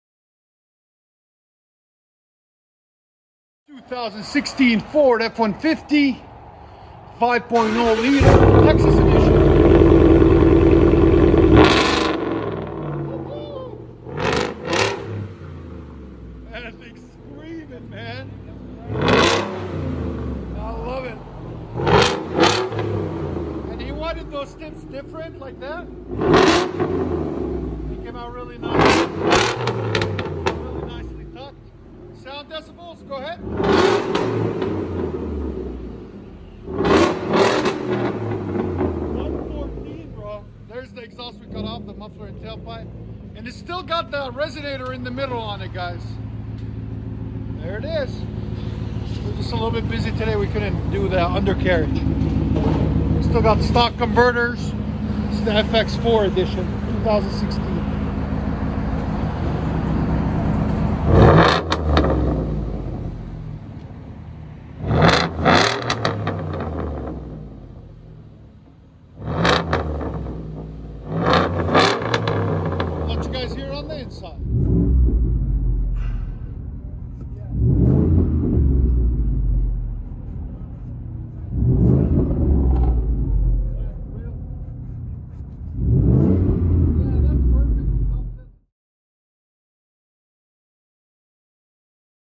2016 Ford F-150 5.0L COYOTE V8 Texas Edition DUAL EXHAUST w/ STRAIGHT PIPES!
2016 Ford F-150 5.0L COYOTE V8 DUAL EXHAUST w/ STRAIGHT PIPES!